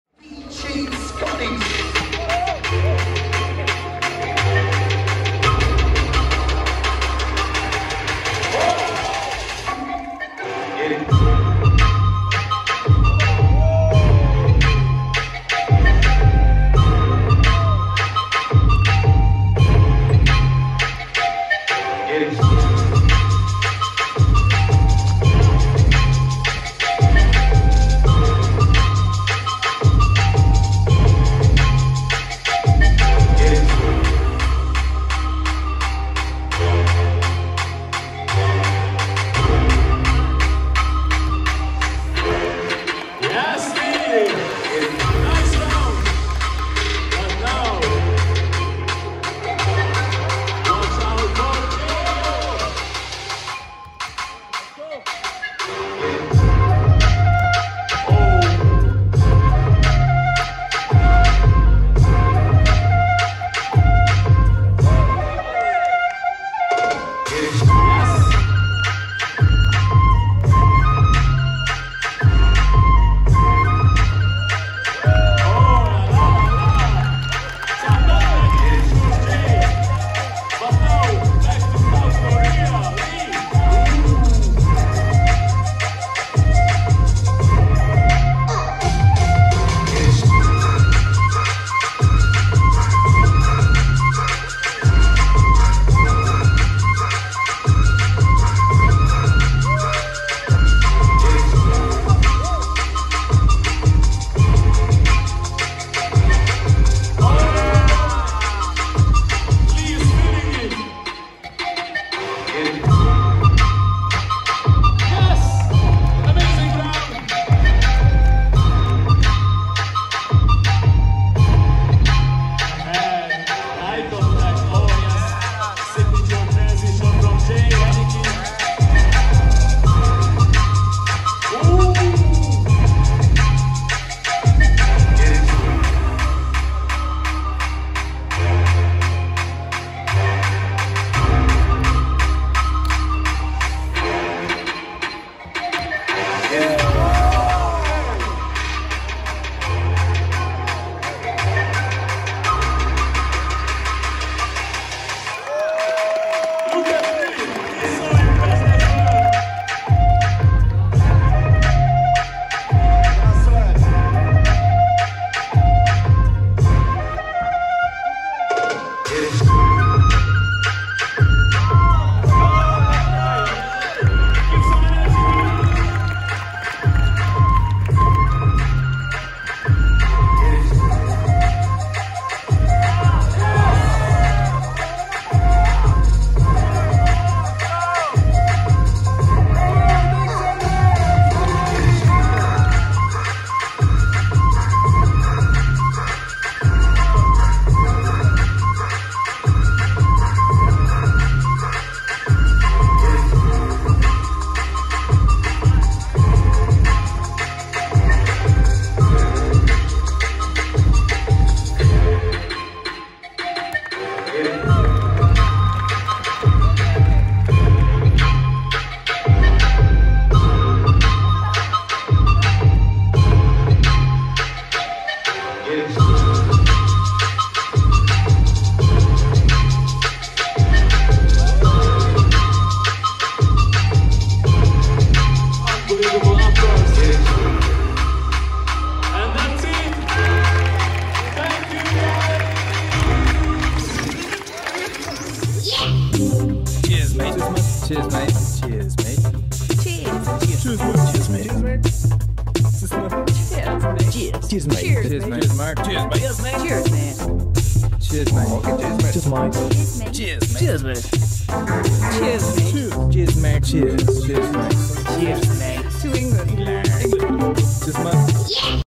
Hylee vs Jay - Top 32 | Super Ball 2022
HyunYong Lee (KOR) vs Jay Hennicke (AUS) - Top 32 | Super Ball Freestyle Football World Open 2022

🏆 Winner: Jay